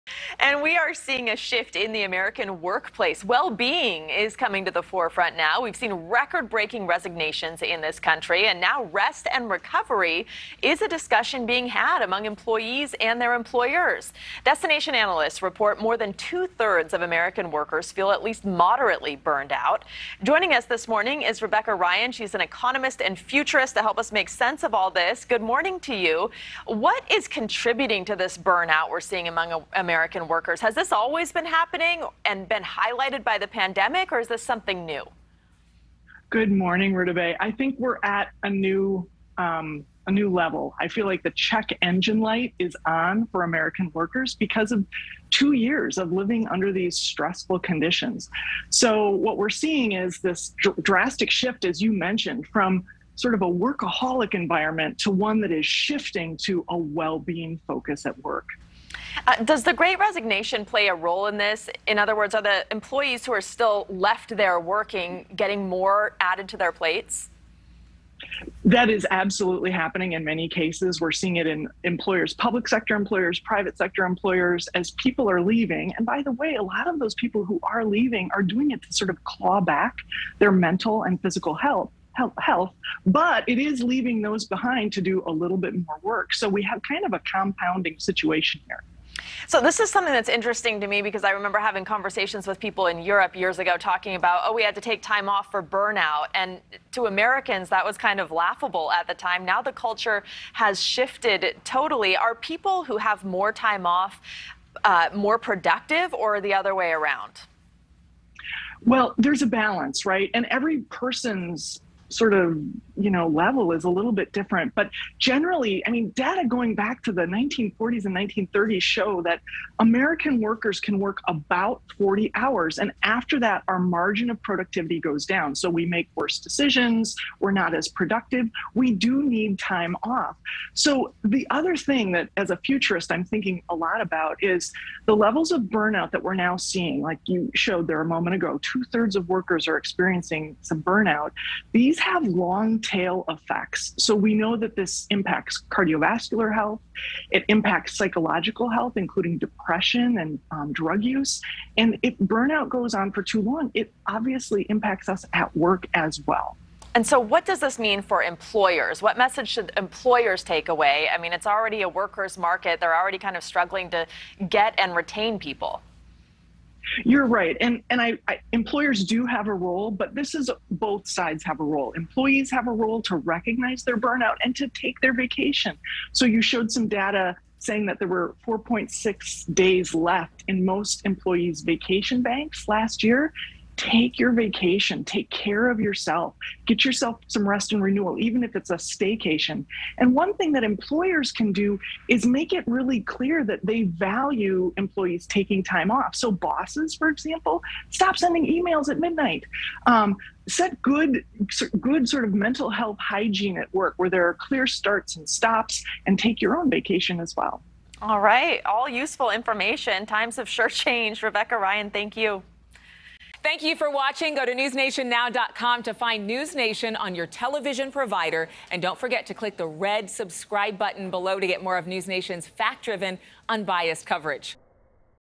Report: Two-thirds of American workers are burned out | Morning in America
Economist and futurist Rebecca Ryan discusses avoiding worker burnout and how employees are not taking enough vacations.